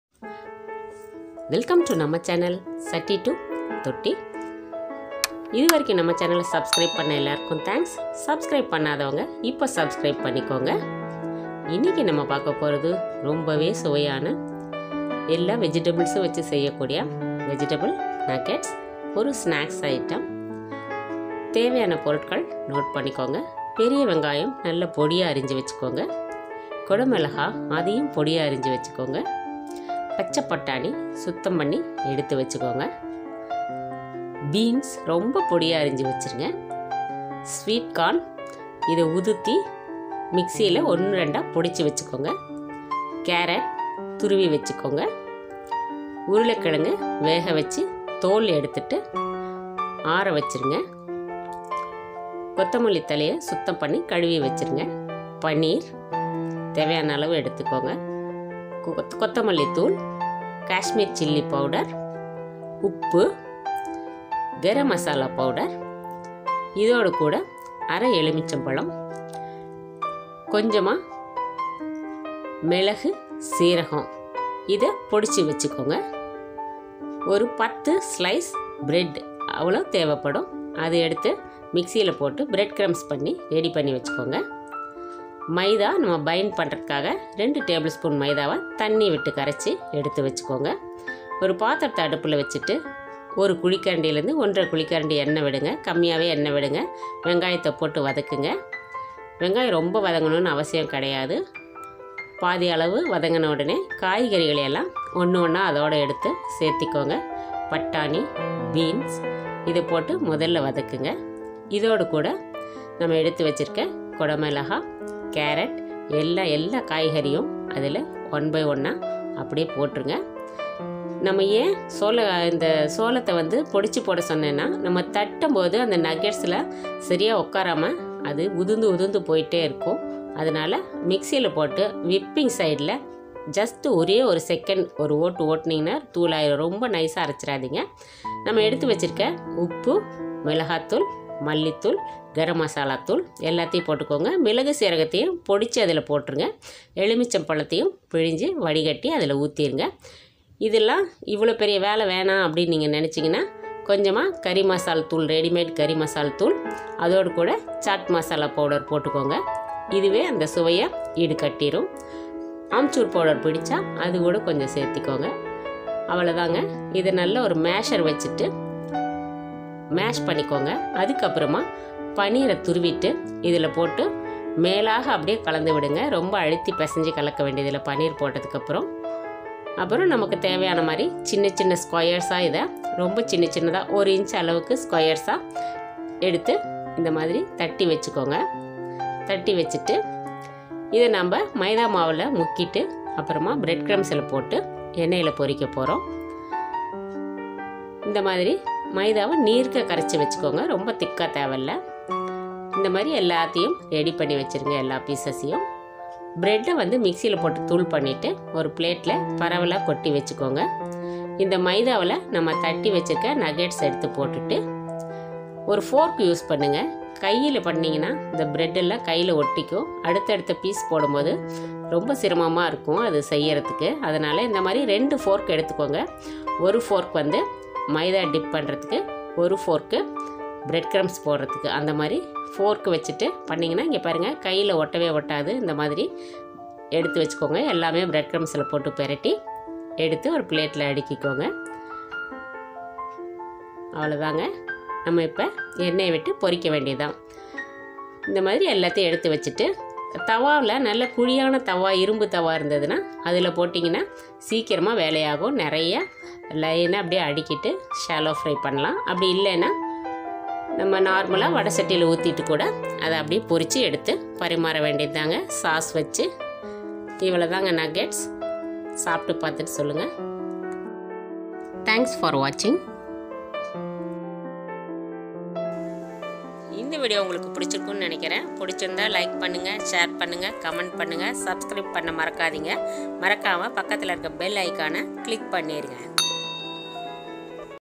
Healthy snacks recipe in tamil | How to make Vegetable Nuggets | Veg nuggets
How to make healthy and tasty vegetable nuggets in tamil | Evening snacks recipe with all vegetables | Tea time snacks recipe | Kids will love this Vegetable nuggets | Veg nuggets using less oil

Ingredients :

onion, green peas, carrot, beans, boiled potato, capsicum,sweet corn , paneer, coriander leaves,
Salt, coriander powder ,kashmir chilli powder, garam masala, (pepper and jeera powder,half lemon squeezed) or (curry masala powder, chat masala and amchur powder)
Bread crumbs, Maida for binding,

Serving suggestions: Serve with Tomato sauce and Mayonesse
(we have used all vegetables you can also try using basic vegeables- potato, carrot, beans, green peas and onion)